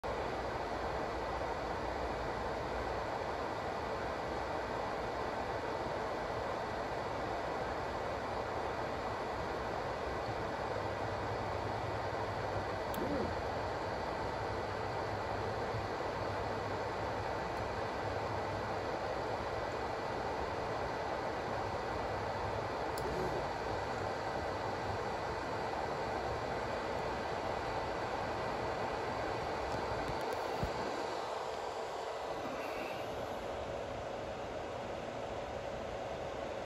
AnyCubic Kobra Neo - Levelling Faild (solution in comments)
AnyCubic Kobra Neo kept failing levelling.
I got it fixed and the solution is in the comments, I just can't seem to pin that comment anymore.